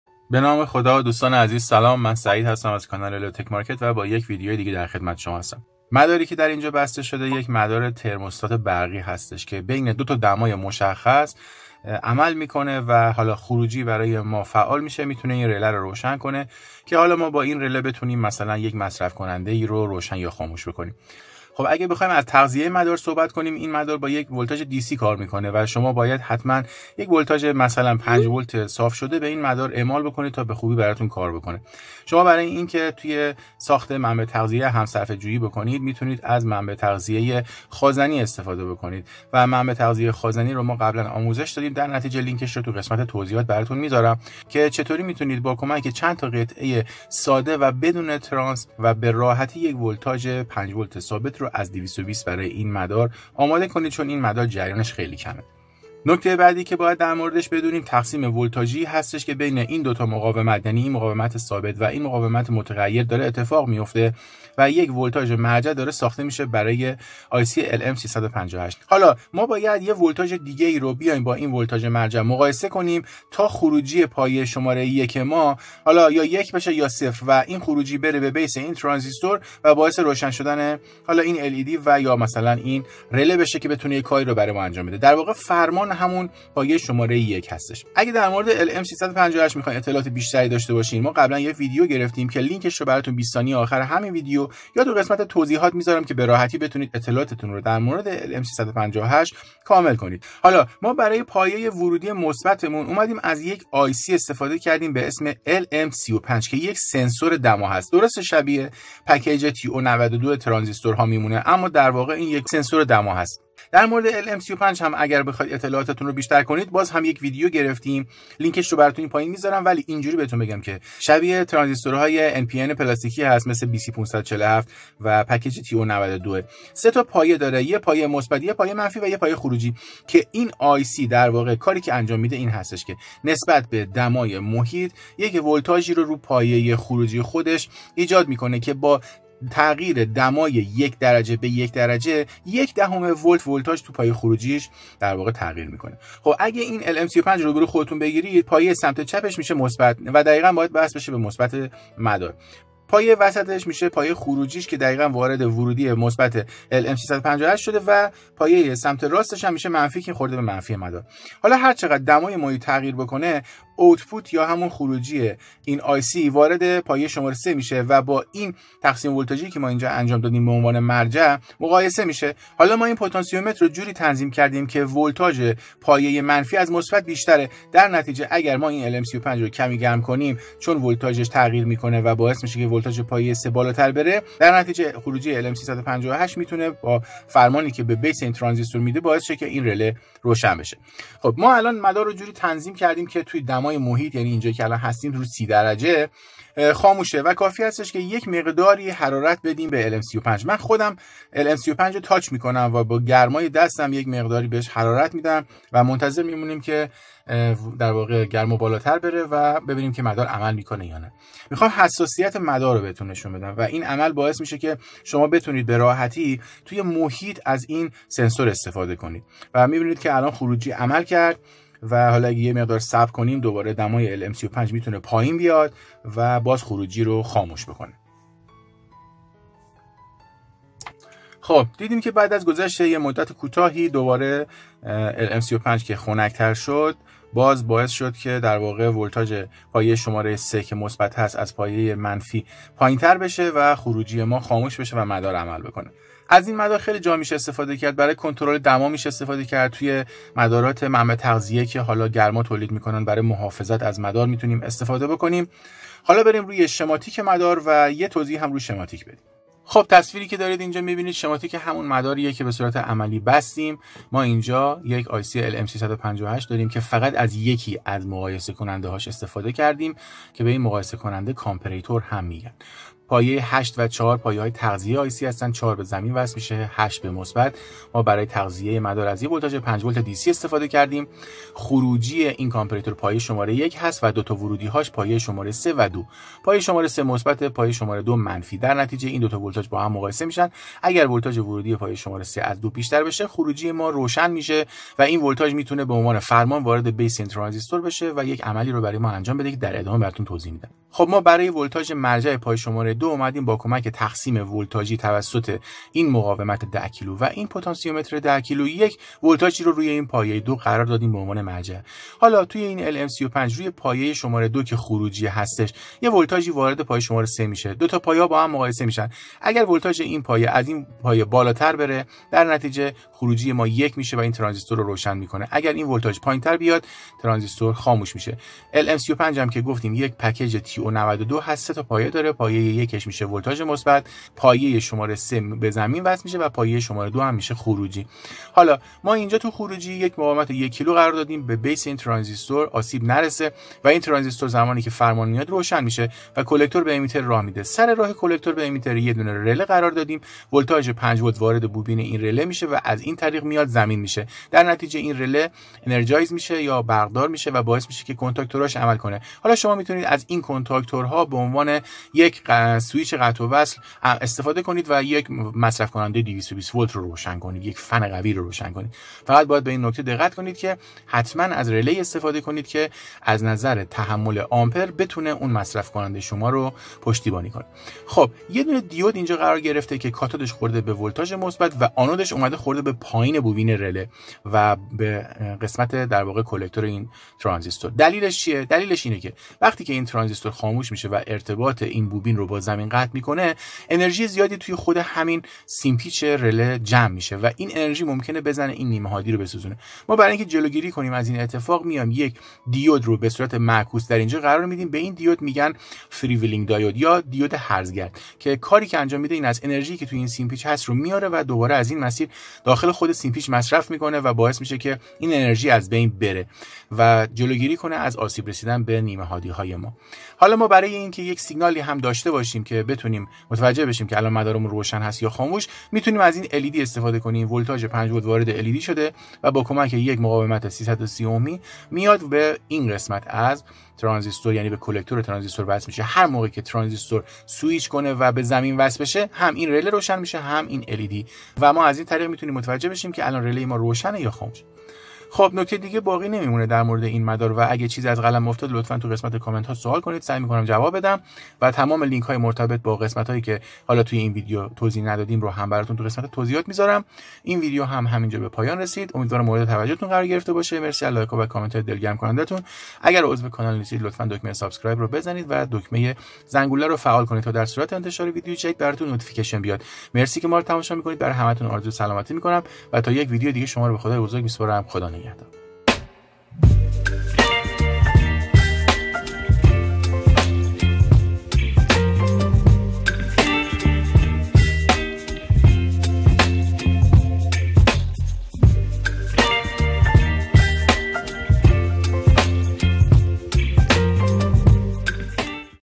آموزش ساخت مدار کنترل دمای محیط با سنسور lm35
با این مدار دمای محیط رو کنترل کن و کولر رو خاموش یا روشن کن